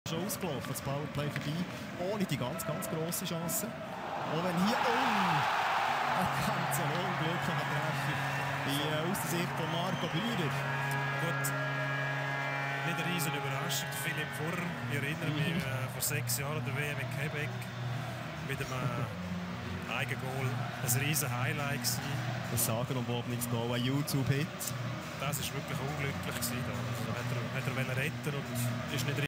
Philippe Furrer scores another classic own goal
2008 in the World Championships in Québec Philppe Furrer scored an amazing own goal. Now he's done it again in the Swiss Cup for Bern.